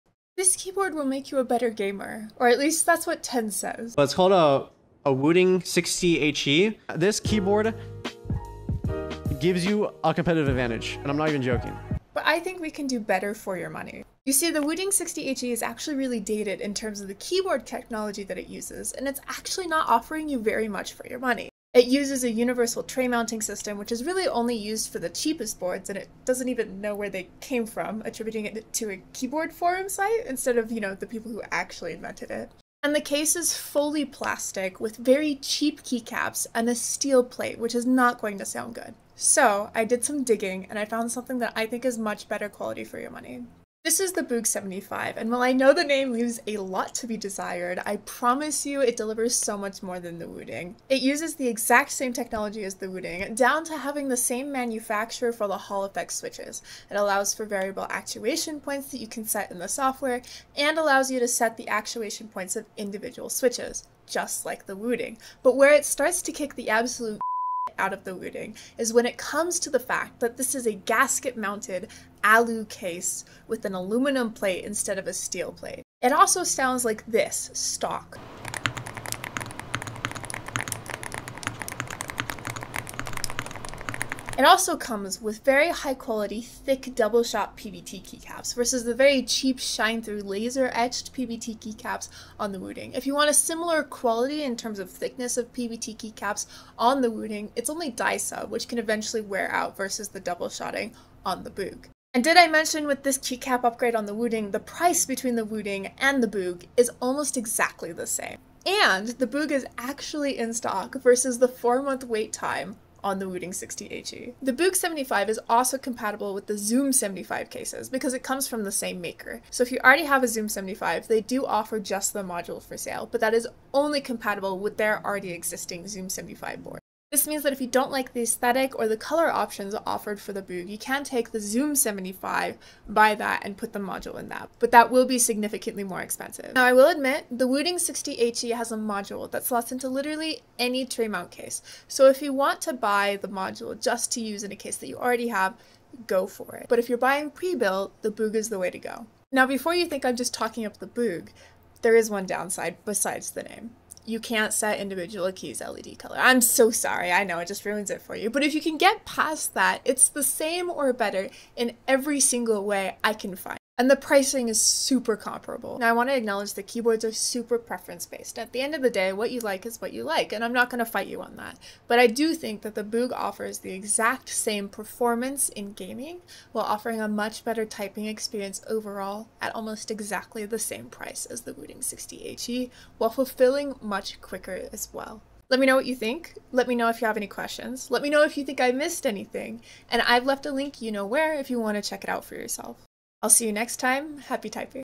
The ACTUAL best gaming keyboard (it's not the Wooting)
Honestly when I asked them to send this to me I did not expect to like it at all (those who watched my first impression unboxing on twitch can attest)- imagine my surprise when I not only liked it, but liked it enough to daily it since I got it in!